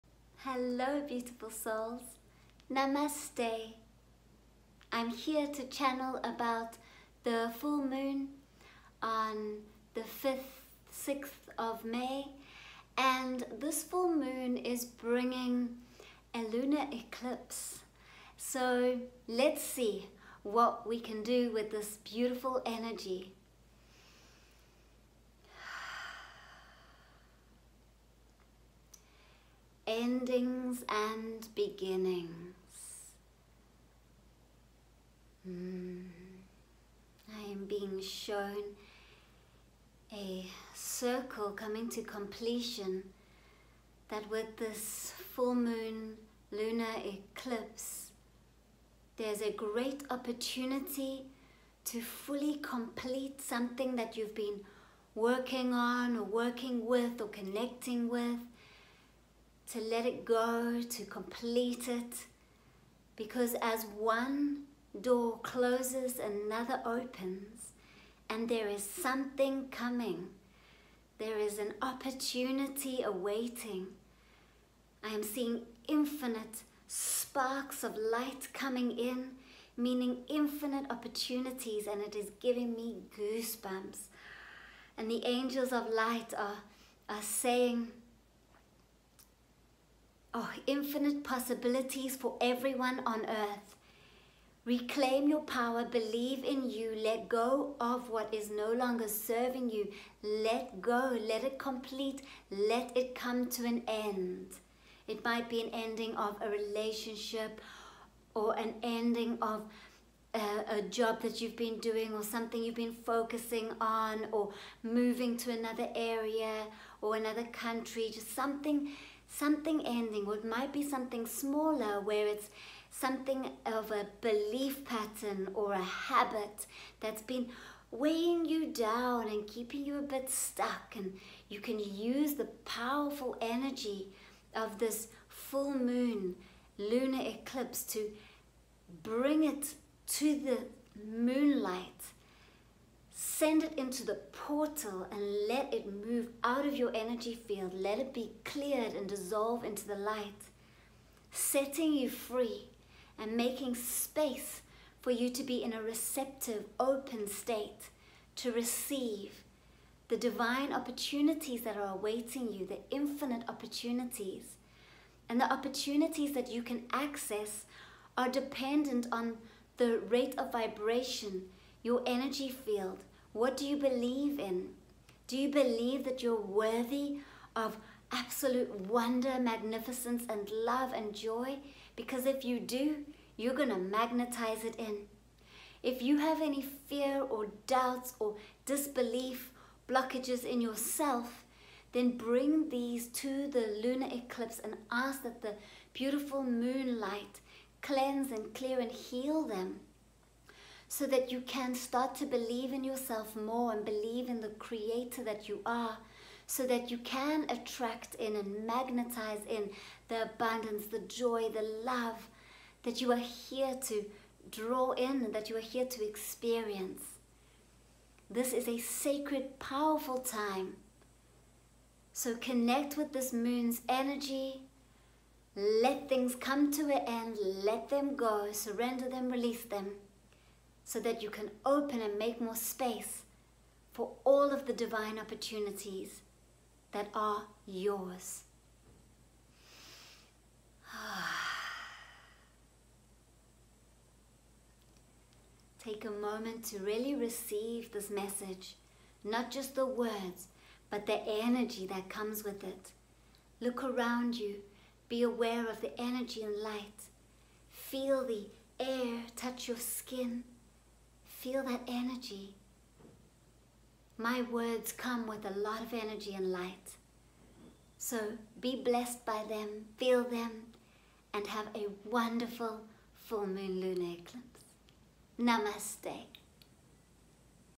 Full moon lunar eclipse in May.Portal of light! Endings & beginnings.full moon#lunareclipse#light
Closure and endings are healthy. They are part of the cycle of life. As one door closes, another opens. You have infinite opportunities awaiting you - don’t stand in your own way! Open to the light.

Thank you beautiful soul.

Namaste 🙏